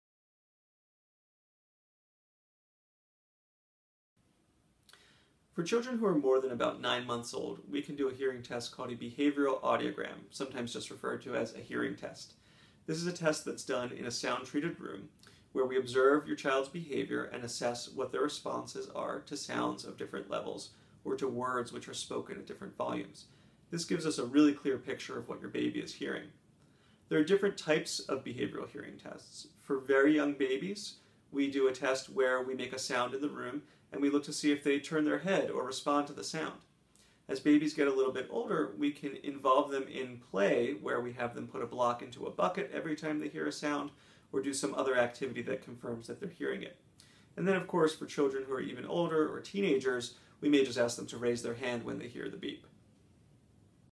Behavioral audiometry: What you need to know
Learn about behavioral audiometry as part of your child's hearing loss testing from Michael Cohen, MD, of MassGeneral Hospital for Children (MGHfC) and the Massachusetts Eye and Ear Infirmary,